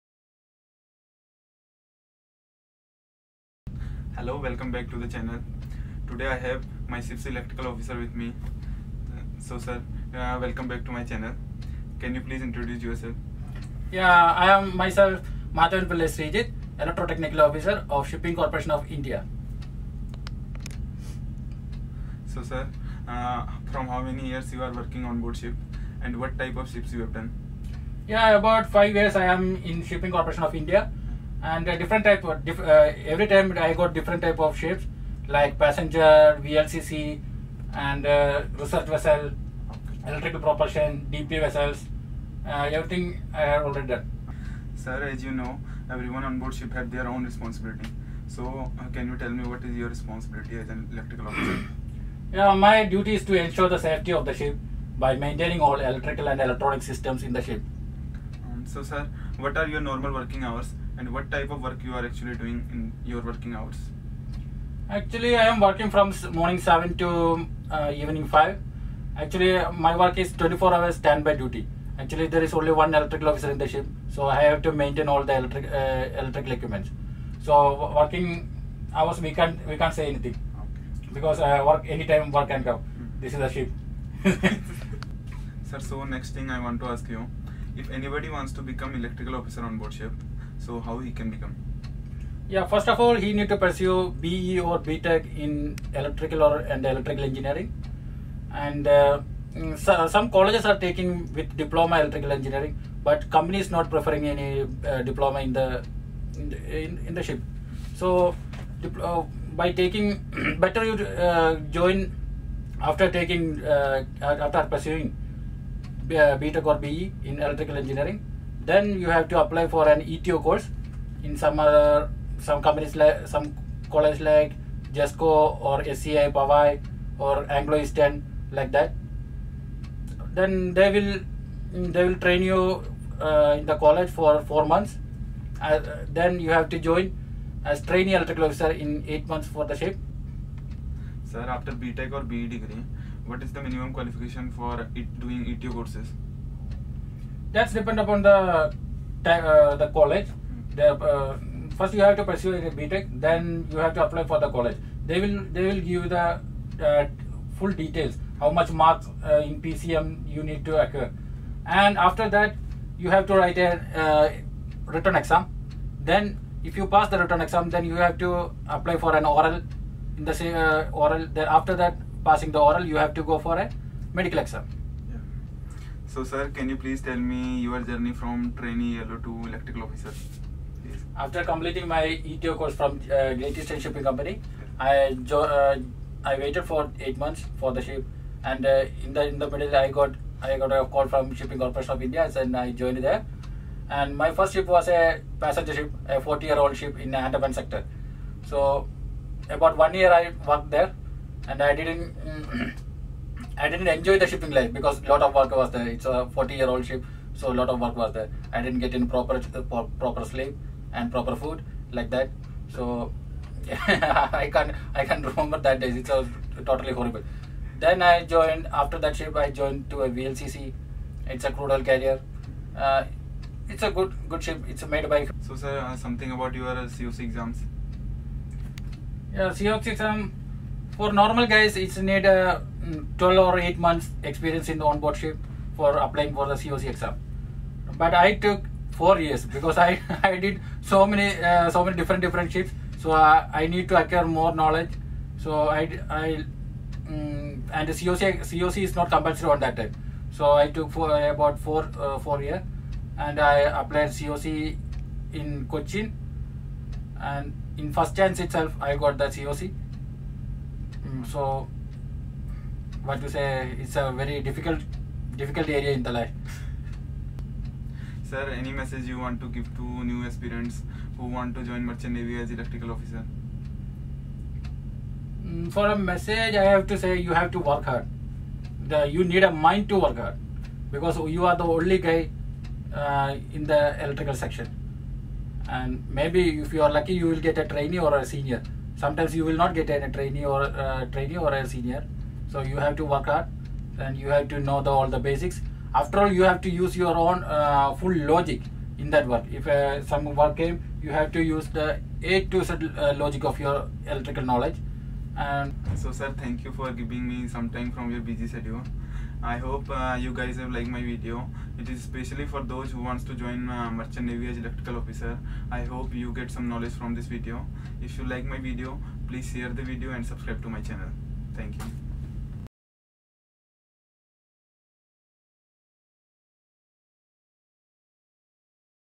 Q and A WITH ELECTRICAL OFFICER ONBOARD SHIP !! BY PANKAJ DIGARI
IN THIS VIDEOS I HAVE Q&A WITH ELECTRICAL OFFICER  ABOUT HIS STRUGGLES IN EARLY DAYS . HIS JOURNEY FROM TRAINEE ELO TO A CERTIFIED ELECTRICAL OFFICER.